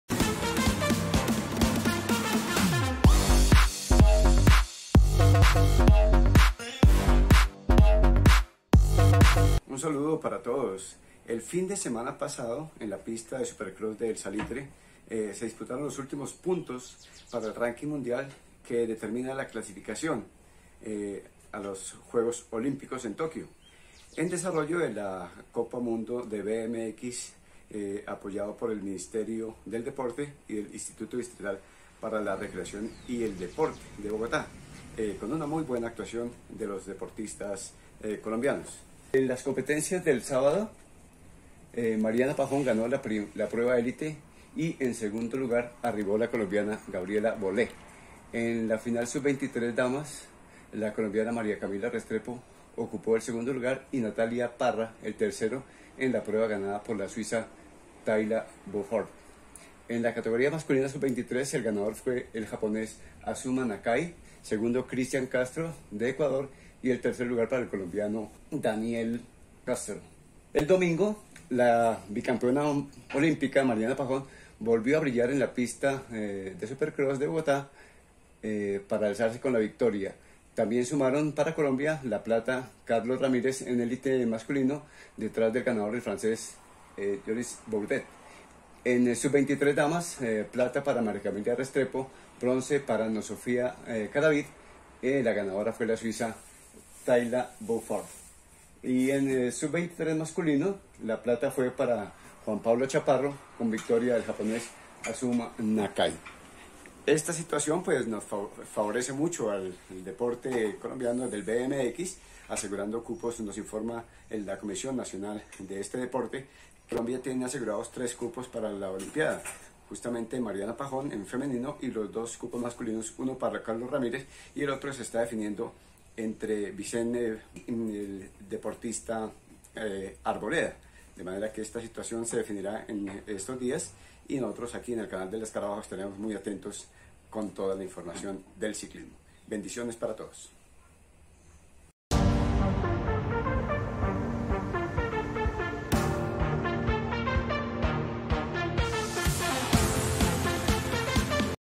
Copa Mundo BMX Bogotá
Mariana Pajón dos veces medalla de oro